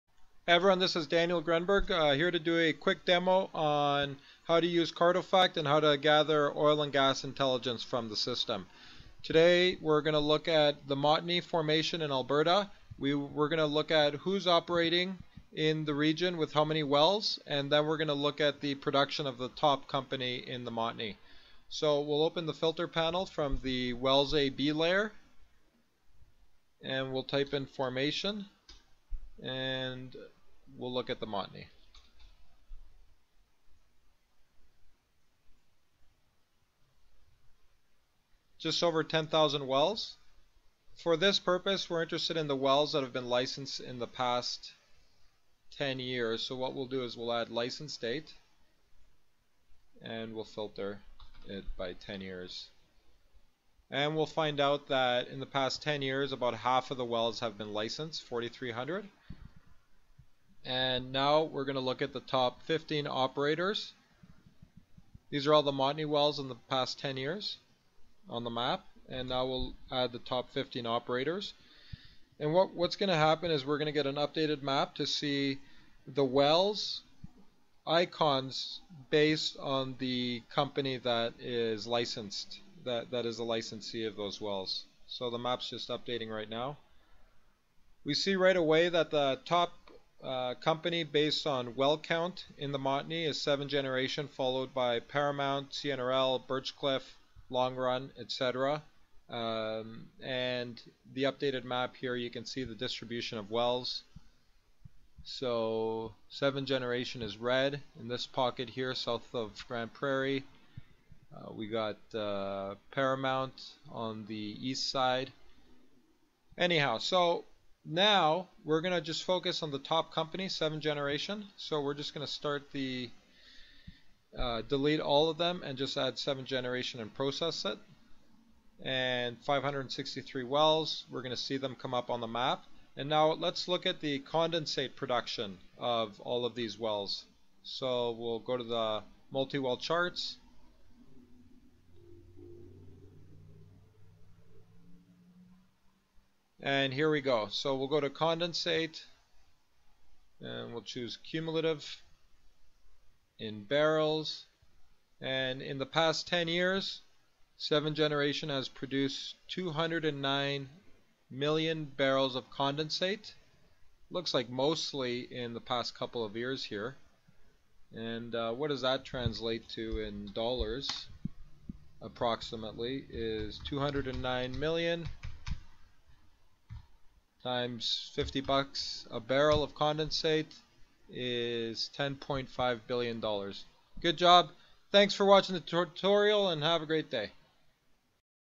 A look at the Montney and Seven Gen's Condensate Prod'n
Using Cartofact to learn about what's going on in the Alberta Montney Formation.  We'll look at the top 15 producers in the area and then we'll look into the condensate production of Seven Generation Energy.